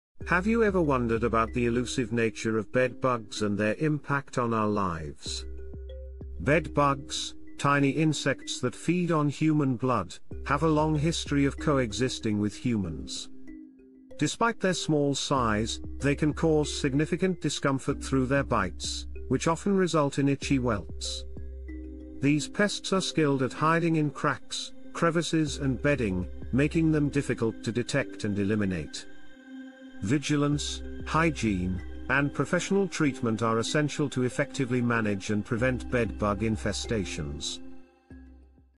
Bed Bugs Exposed: The Hidden Impact on Our Everyday Lives
Bed bugs, tiny insects that feed on human blood, have a long history of coexisting with humans. Despite their small size, they can cause significant discomfort through their bites, which often result in itchy welts. These pests are skilled at hiding in cracks, crevices, and bedding, making them difficult to detect and eliminate. Vigilance, hygiene, and professional treatment are essential to effectively manage and prevent bed bug infestations.